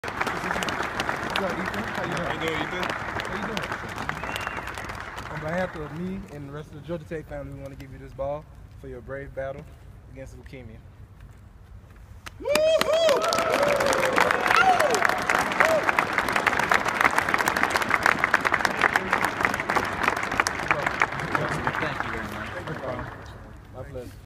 Georgia Tech Football honors Ethan Young
Ethan Young is four-year-old Georgia Tech fan battling leukemia. His 10-year-old sister is donation her bone marrow for a transplant. The Young family attended Georgia Tech practice on Thanksgiving morning, and Ethan was presented with an autographed football by the team's captains -- David Sims, Jeremiah Attaochu and Jemea Thomas. Go Jackets!